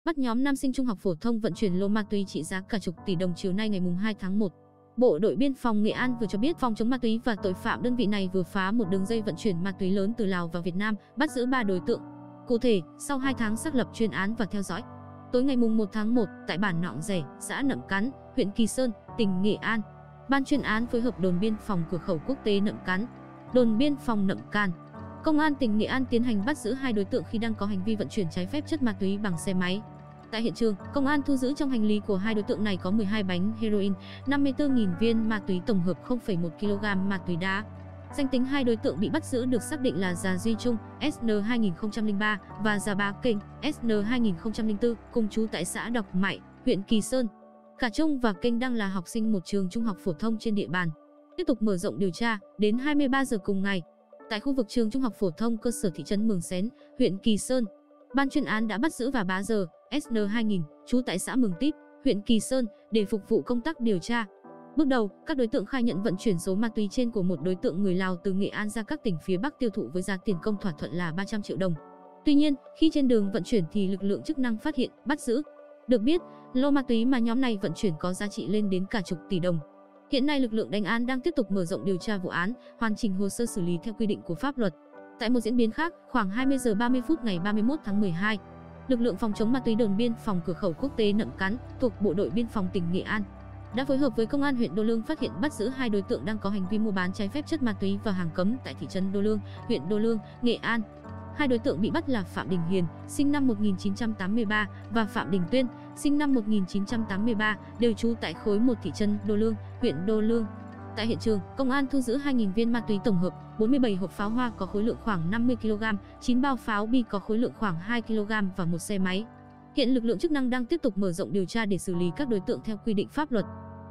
Nam học sinh vận chuyển m.a t.úy trị giá chục tỷ đồng | Đường dây vận chuyển từ Lào
Chiều nay ngày 2/1, Bộ Đội Biên phòng Nghệ An vừa cho biết, Phòng Phòng chống ma túy và tội phạm đơn vị này vừa phá một đường dây vận chuyển ma túy lớn từ Lào vào Việt Nam, bắt giữ 3 đối tượng.
Cụ thể, sau 2 tháng xác lập chuyên án và theo dõi, tối ngày 1/1, tại bản Noọng Dẻ (xã Nậm Cắn, huyện Kỳ Sơn, tỉnh Nghệ An) Ban chuyên án phối hợp Đồn Biên phòng Cửa khẩu quốc tế Nậm Cắn, Đồn Biên phòng Nậm Càn, Công an tỉnh Nghệ An tiến hành bắt giữ 2 đối tượng khi đang có hành vi vận chuyển trái phép chất ma túy bằng xe máy. Tại hiện trường, công an thu giữ trong hành lý của 2 đối tượng này có 12 bánh heroin, 54.000 viên ma túy tổng hợp, 1 kg ma túy đá.